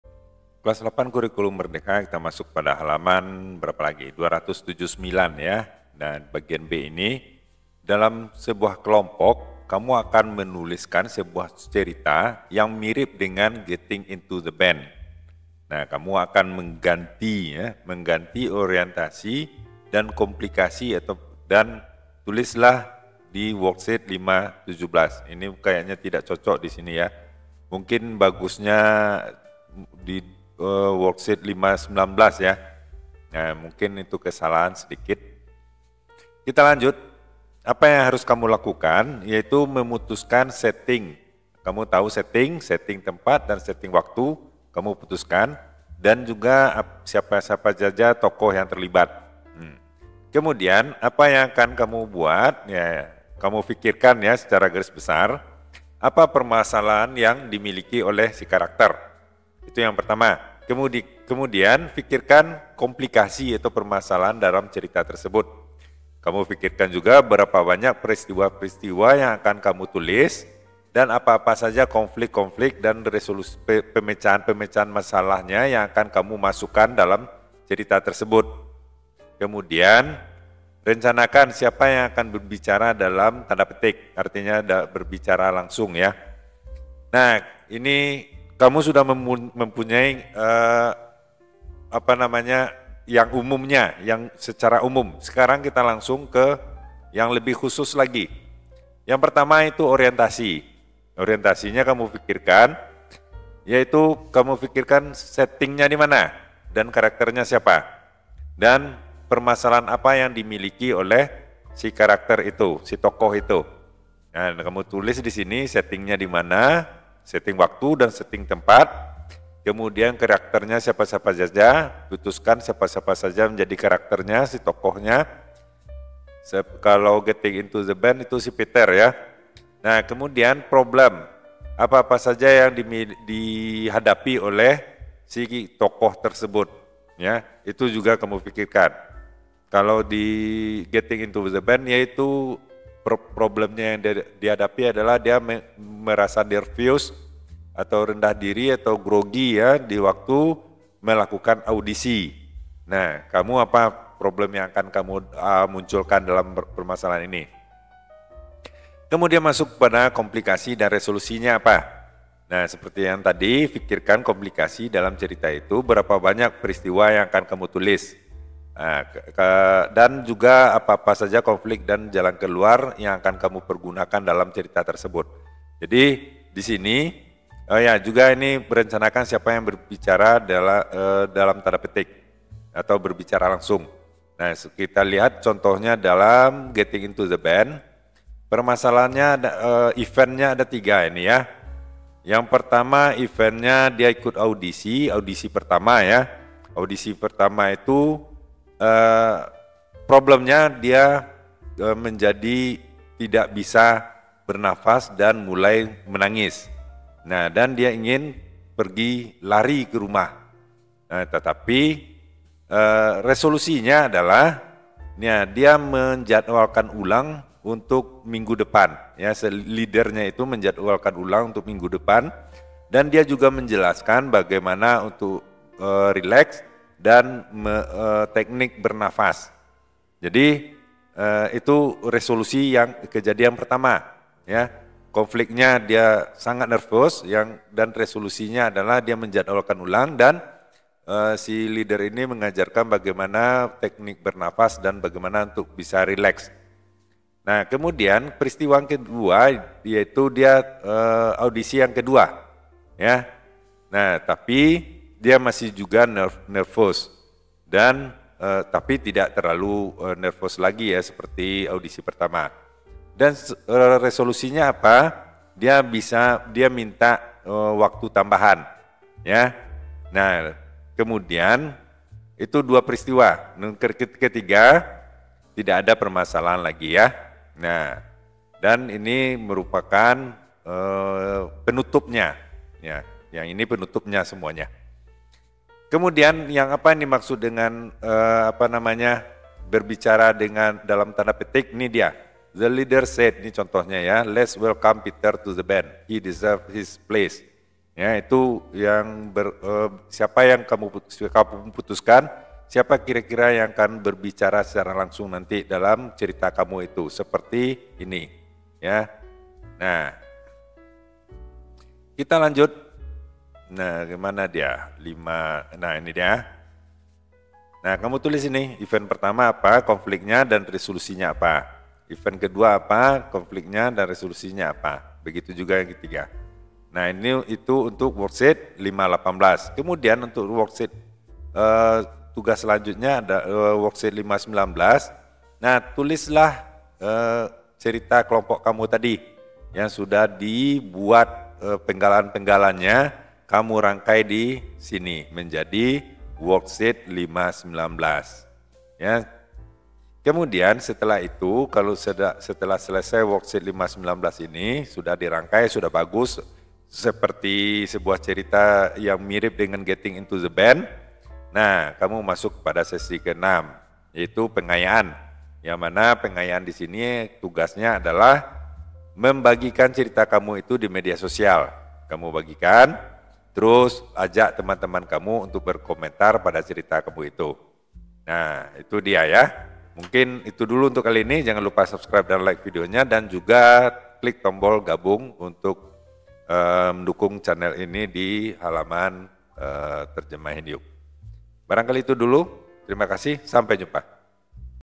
Hal. 279-280 kelas 8 Terjemahan Buku Bahasa Inggris SMP Kurikulum Merdeka
Write a story similar to Getting into the Band and change the orientation and the complication. Write your work in  Worksheet 5.18 and worksheet 5.19 .
Decide the settings and characters, orientation and the events wilol you write about.
terjemahin yuk
terjemahinyuk
terjemahkan yuk
english class 8
Kelas VIII
English for Nusantara
Tingkat Lanjut
kunci jawaban
belajar bahasa inggris
pelajaran
halaman